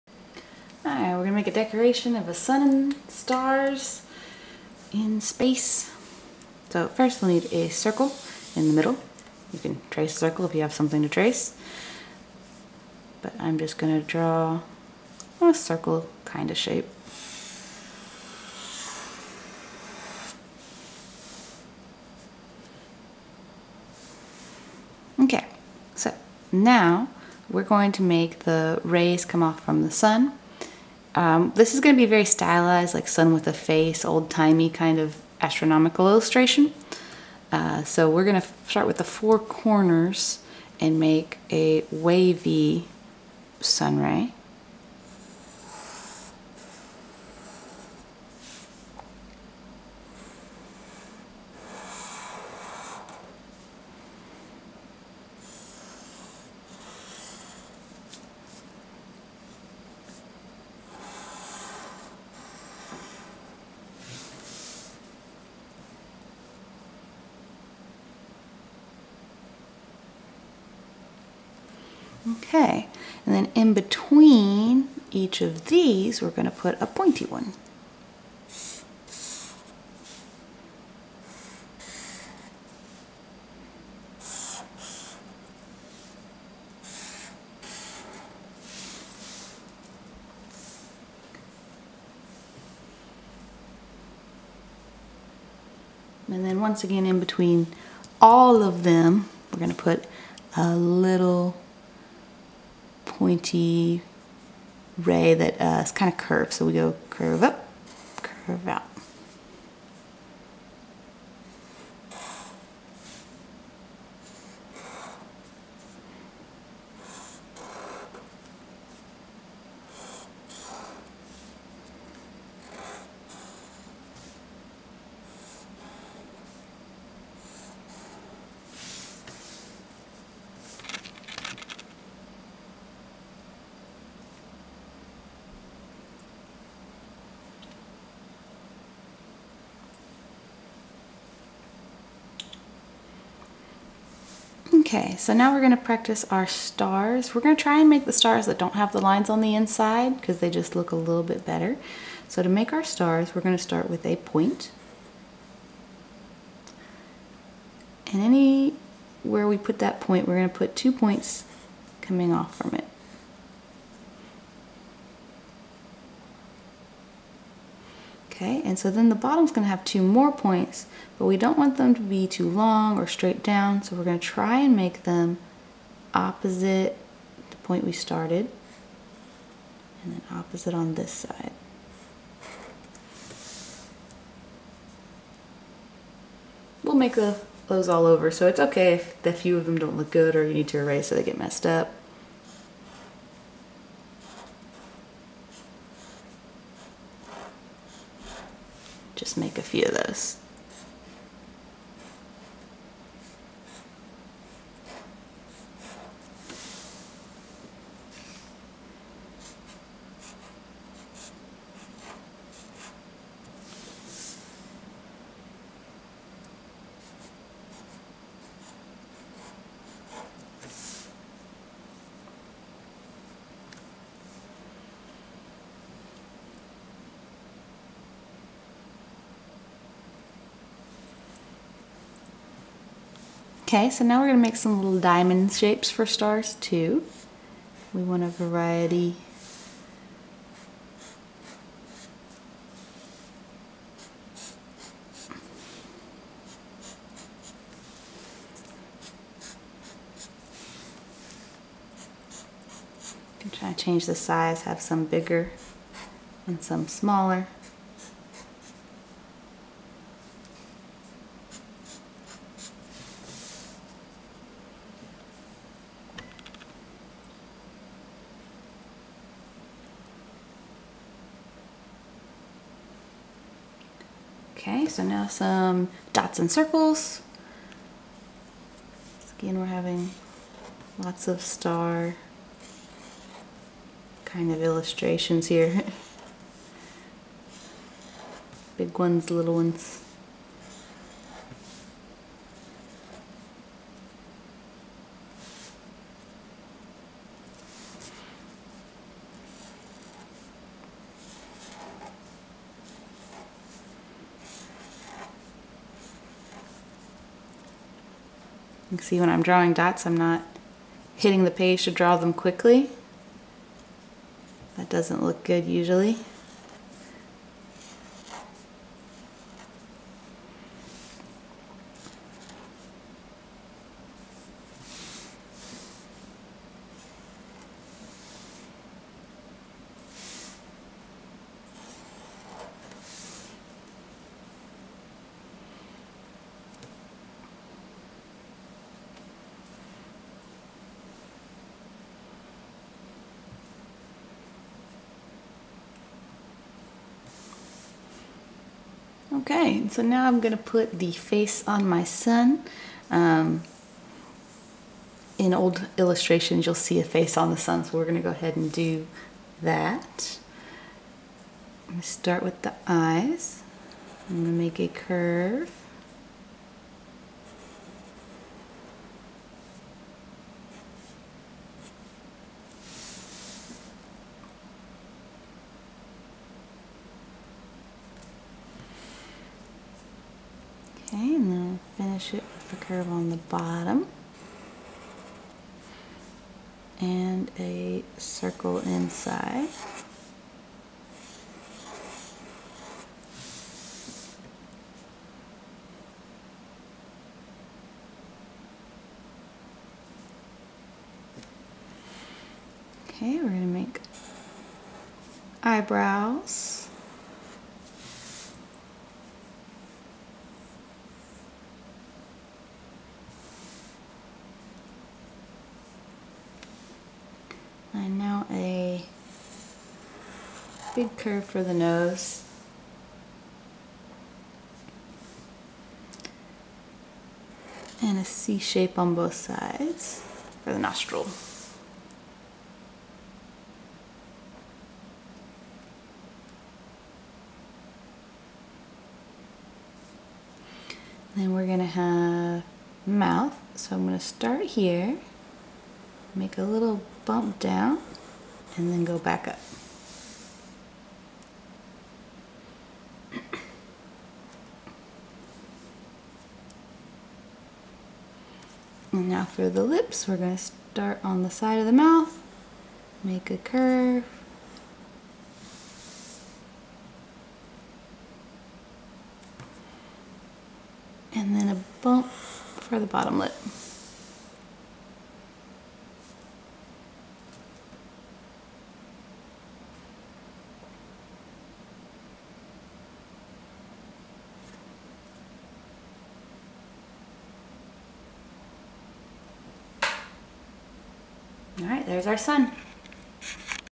Step by step drawing a sun with a face
best for intermediate artists 3rd grade and up.